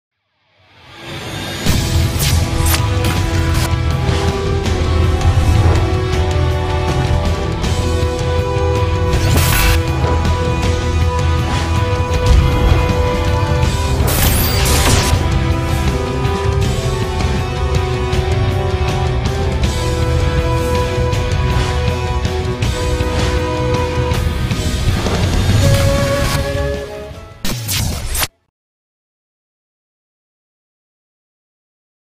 Channel opening test out
I decided to try and give an opening for my channel. This is my first one so dont judge too harshly of it. Enjoy :D